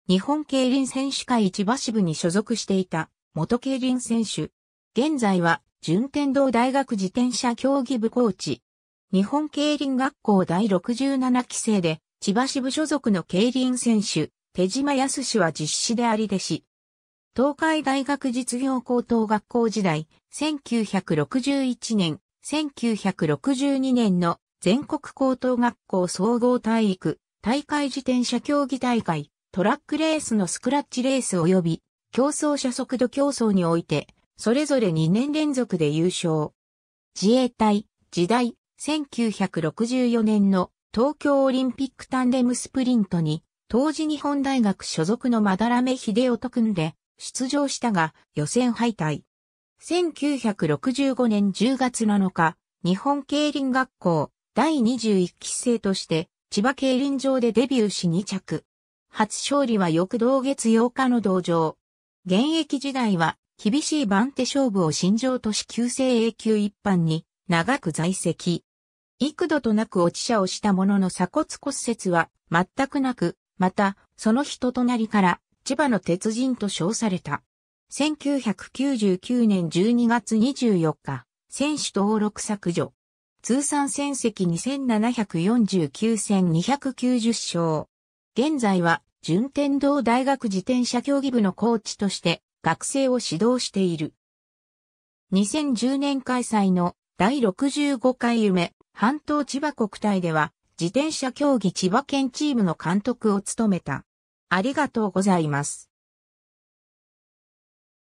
手嶋敏光
手嶋 敏光（てしま としみつ、1942年12月12日 - ）は、日本競輪選手会千葉支部に所属していた元競輪選手。
現在は順天堂大学自転車競技部コーチ。
日本競輪学校第67期生で、千葉支部所属の競輪選手、手嶋靖は実子であり弟子。
 東海大学実業高等学校（1979年に廃校）時代、1961年、1962年の全国高等学校総合体育大会自転車競技大会・トラックレースのスクラッチレース及び競走車速度競走において、それぞれ2年連続で優勝。
 自衛隊時代、1964年の東京オリンピック・タンデムスプリントに、当時日本大学所属の班目秀雄と組んで出場したが、予選敗退。
 1965年10月7日、日本競輪学校第21期生として、千葉競輪場でデビューし2着。
初勝利は翌同月8日の同場。
現役時代は厳しい番手勝負を信条とし旧制A級1班（現在のS１）に長く在籍。
幾度となく落車をしたものの鎖骨骨折は全く無く、また、その人となりから『千葉の鉄人』と称された。
 1999年12月24日、選手登録削除。
通算戦績2749戦290勝。
 現在は順天堂大学自転車競技部のコーチとして学生を指導している。
2010年開催の第65回ゆめ半島千葉国体では、自転車競技千葉県チームの監督を務めた。